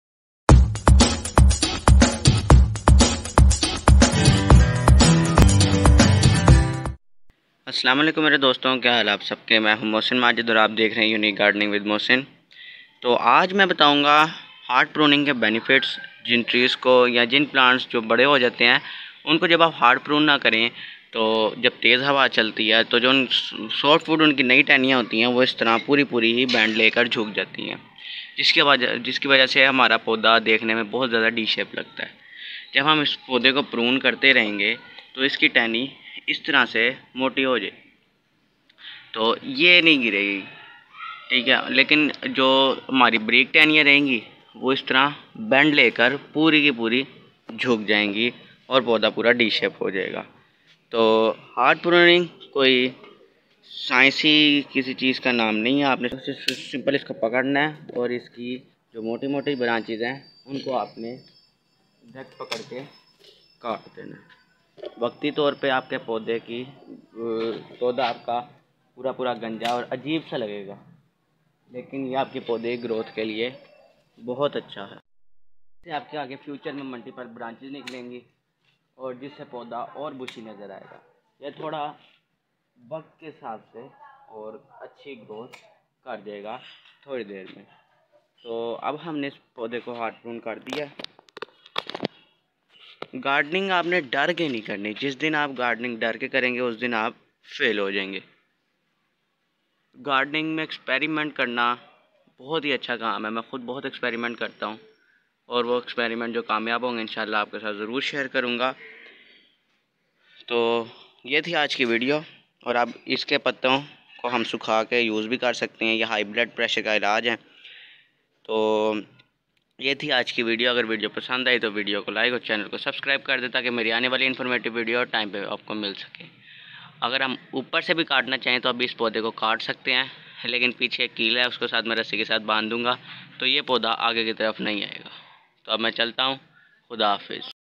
Hard pruning of plant|| plant ko hard prun Kesay krain ||Hard pruning benefits
today I tell about u how to hard prun a plant or tree and hard pruning benefits of plant
secret growth started of plant .
ajj main app ko bataon ga k plant ko hard prun krny ka kayak fida hay .